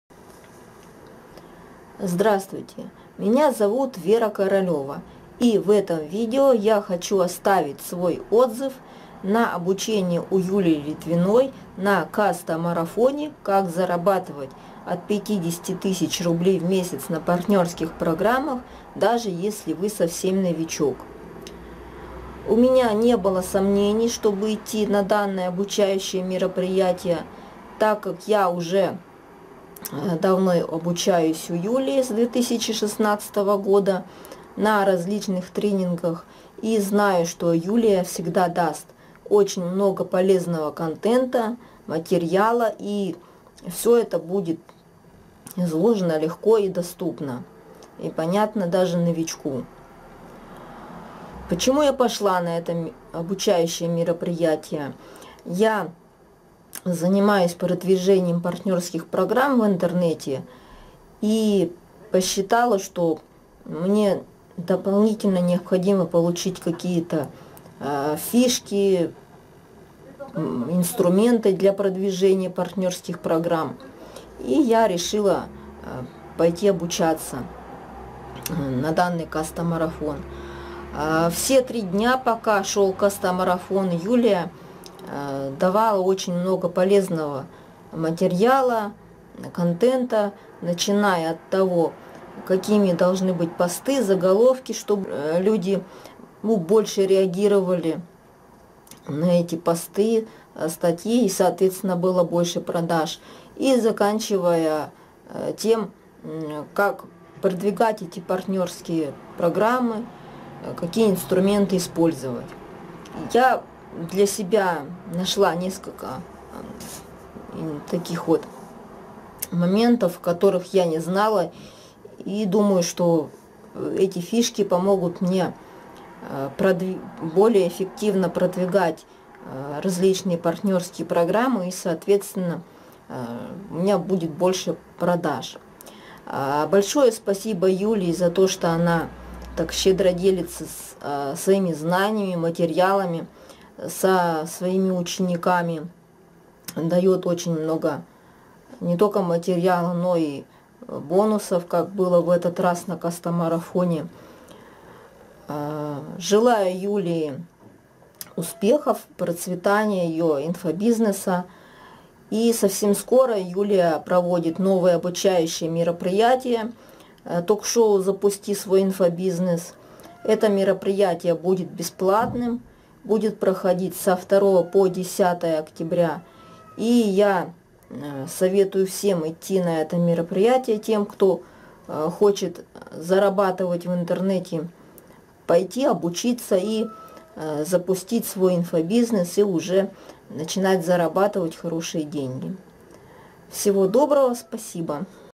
Отзыв Веры Королевой на обучение у Юлии Литвиной на кастамарафоне по партнерским программам
Нажмите ЕЩЁ, чтобы прочитать описание
Мой видеоотзыв на обучение у Юлии Литвиной